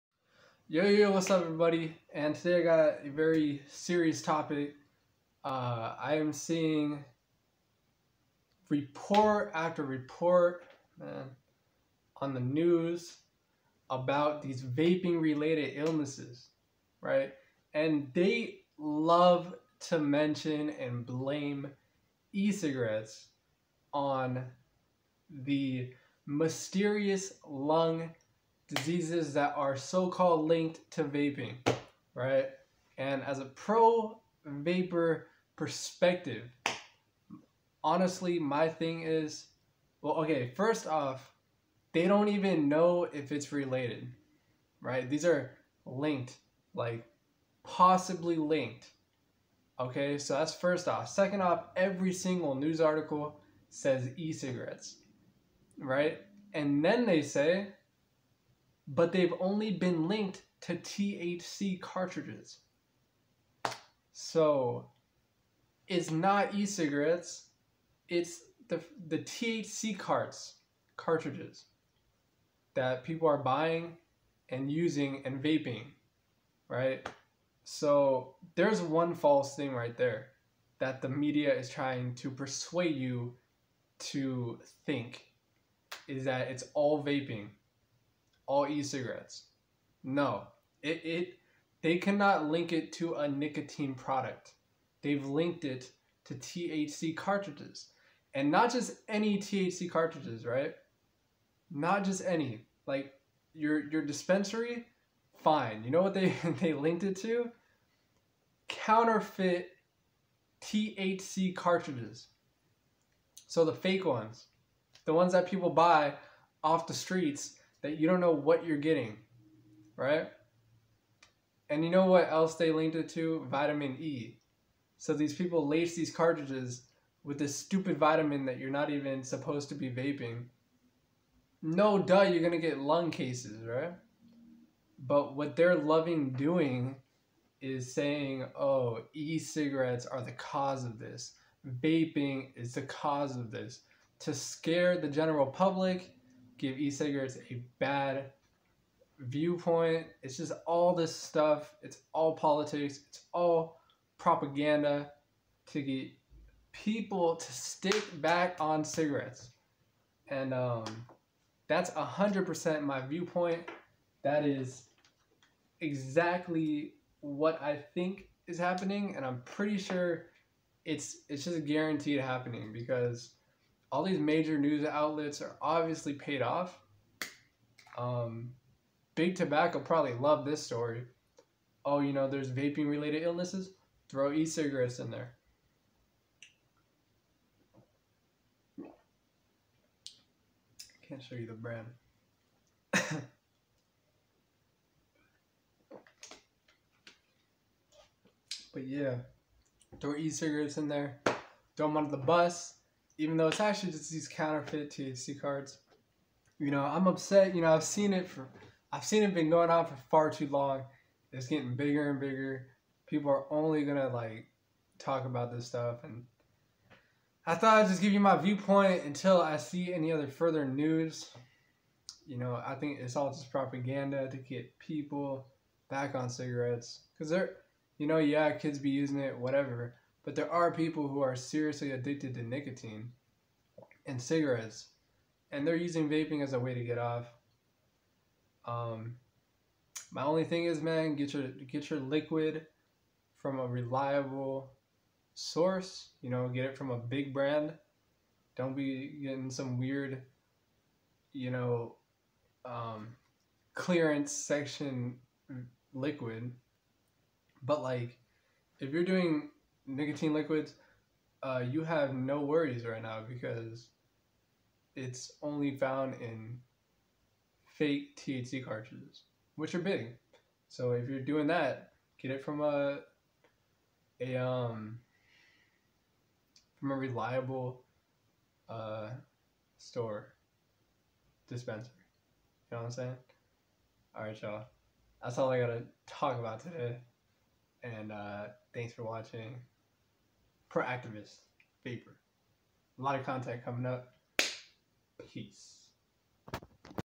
Pro-Vaper Speaks out on New "Vaping-Related" Lung Illnesses
Hear what a vaping activist has to stay about the new lung illnesses being linked to e cigarettes and Vaping.

Scientific government reports new vaping related illnesses and death of three people, fourth being looked into.